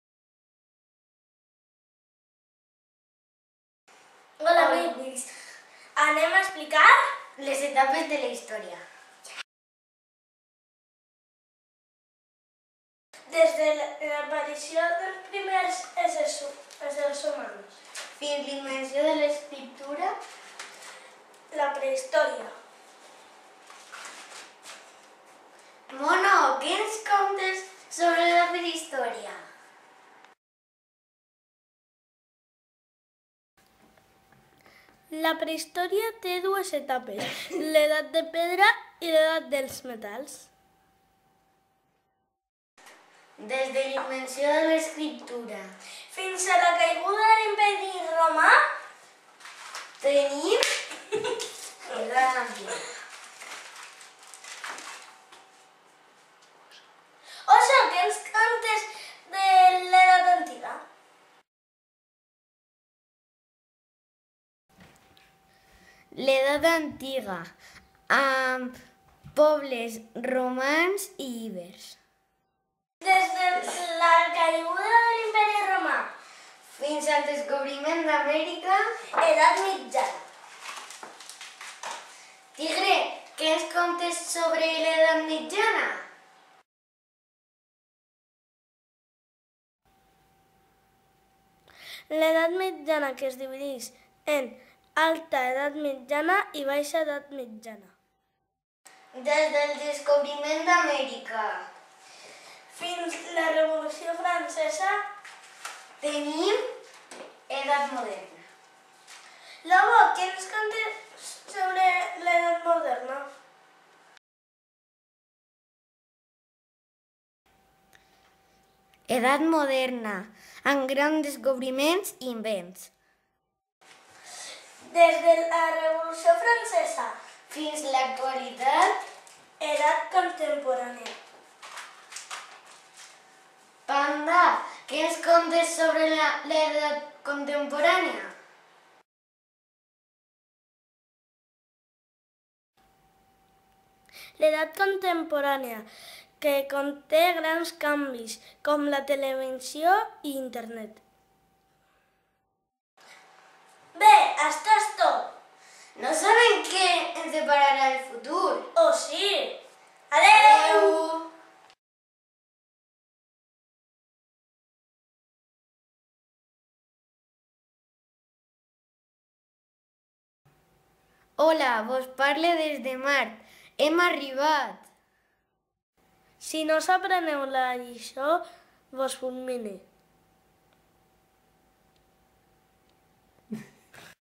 49.SamboriMICE2017INFIPRIM-"LES ETAPES DE LA HISTÒRIA"-CEIP LA VALLVERDA VALLVERDA BAIXA- ELX
Vídeo explicatiu sobre les etapes de la història realitzat en l'àrea de ciències socials per dos alumnes de 2n de primària del CEIP La Vallverda.